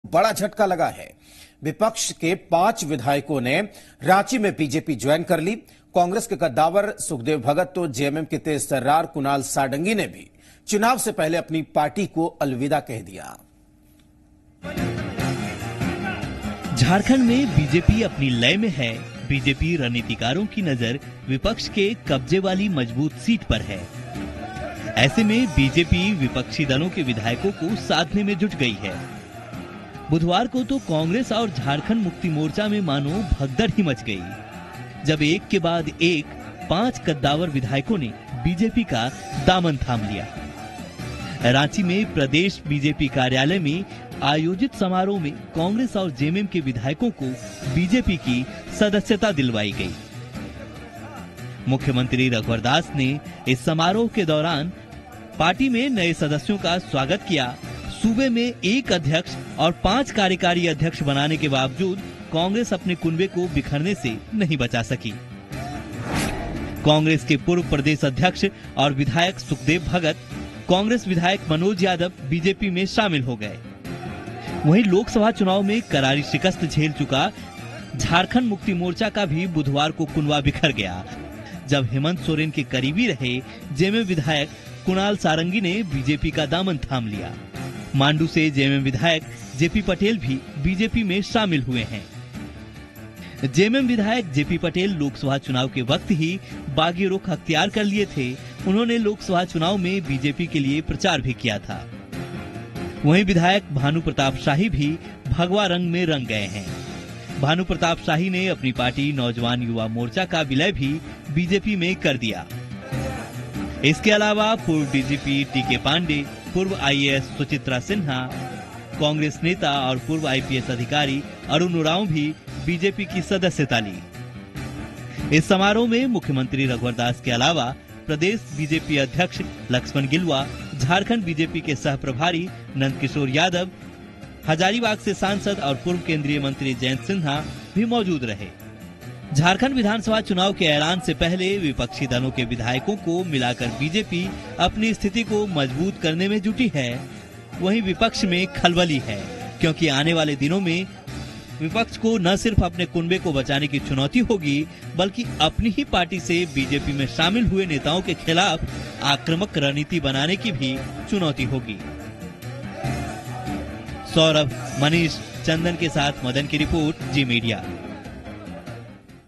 झारखंड: BJP ने विपक्ष को दिया 'सॉलिड झटका'
दिवाली से पहले झारखंड मुक्ति मोर्चा और कांग्रेस को बड़ा झटका लगा है. विपक्ष के 5 विधायकों ने आज रांची में बीजेपी ज्वाइन कर लिया. कांग्रेस के कद्दावर सुखदेव भगत तो JMM के तेज-तर्रार कुणाल षाड़ंगी ने भी चुनाव से पहले अपनी-अपनी पार्टी को अलविदा कर दिया.